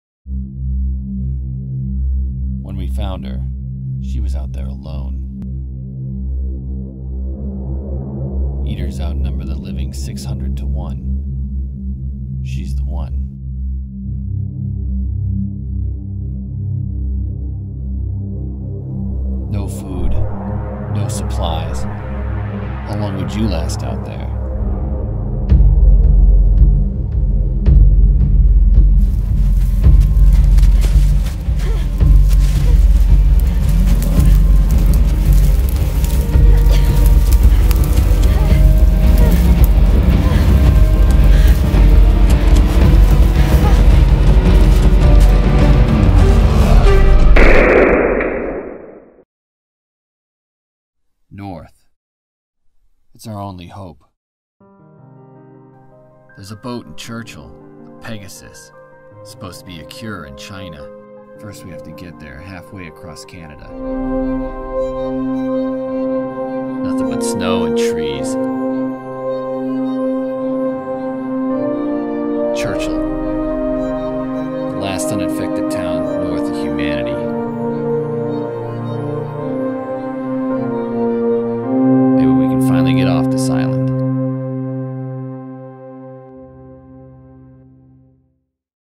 Book Trailer for Dust Eaters North by Ronan Cray
Dust Eaters North is the first novel in the Compass Series, the new post-apocalyptic zombie adventure by Ronan Cray.

“You aren't lucky enough to die.” As the dead feed, an ill-equipped, untrained, desperate band of stragglers trudge 1700 miles north to Churchill, Manitoba. Rumors of a cure in China give them hope, but getting there after the world collapsed isn’t easy. Dead villages, wild animals, unusual locals, -40 degree wind chill, and a lack of transportation pose obstacles even when Eaters aren’t present, but Eaters outnumber the living 600:1.

Come along on a journey across continents in Cray’s new Compass Series. Hopeful and horrifying, startling and relentless, Dust Eaters will haunt you long after you turn the last page. Dust Eaters delivers!

This trailer, like the book, was three years in the making. The original footage was shot back when the book was little more than a dream. Just before I finished the book, I took a trip to Churchill, capturing some great train footage along the way. While I didn’t encounter any zombies, I did eat moose and dogsled through the taiga.

A big thank you to everyone who took part in the original production:

The hatchet job editing was done by me with immense apologies to the magnificent crew above who are far more talented than I am.